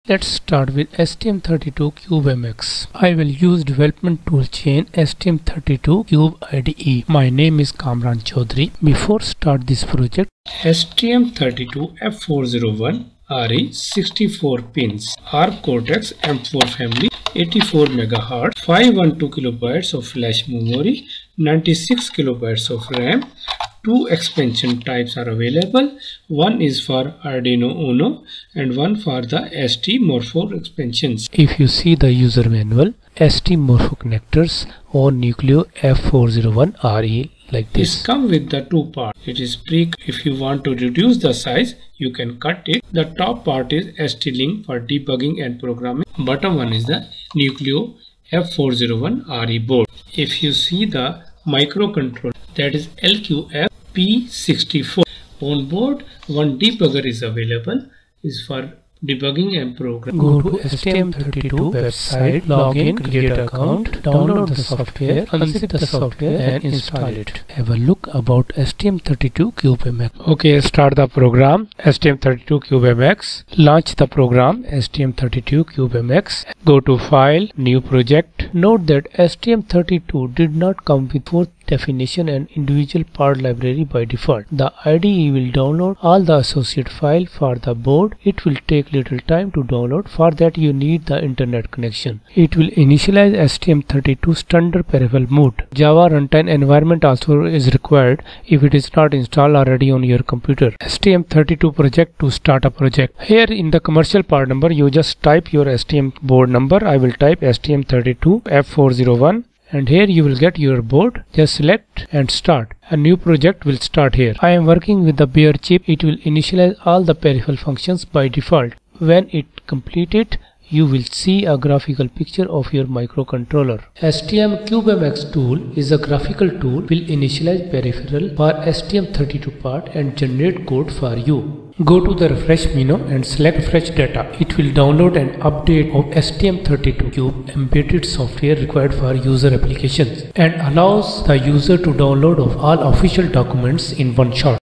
STM32 CubeMX, Cube IDE, Binky LED , F401RE, pin out configuration and debugging
STM32 Nucleo boards setup
Easy steps fast smart
Blink LED Blinking LED with STM32CubeMX,STM32CubeIDE,STM32,STM32 Arm, F401RE,STM32 Nucleo boards
Tools installation
install STM32CubeMX
Install STM32CubeIDE
Download STM32CubeL4 Firmware package
launch STM32CubeMX
start project
select MCU board
Pin out Configuration
RCC  - HSE  - Bypass to Enable PIn PH0/PH1
LSE  -- Crystal/Ceramic Resonator To Enable PC-14/PC-15
SYS core  - debug serial wire to Enable PA-13/PA-14
pinout View
Enable PIN-5 GPIO_Output For on Board LED
Clock configuration
only change HCLK (MHz) to 84 press enter
project manager
give name
select  - Tool chain
Generate project
open project (STM32CubeIDE)
Core - Src -main.C
type code
while (1)
    /* USER CODE END WHILE */

    /* USER CODE BEGIN 3 */
   HAL_GPIO_TogglePin (GPIOA, GPIO_PIN_5);
   HAL_Delay (1000);
save
Debug
connect your board USB
Run
that it